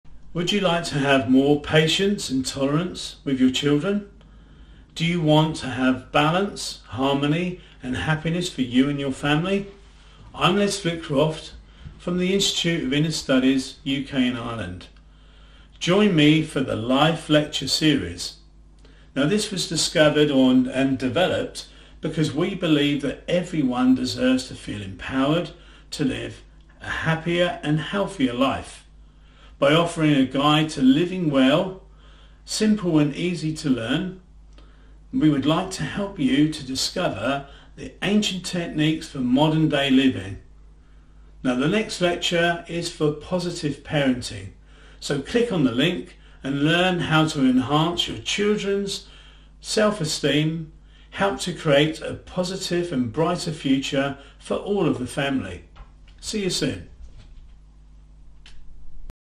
Introduction to Positive Parenting Lecture
Introduction to Positive Parenting  Lecture by Les Flitcroft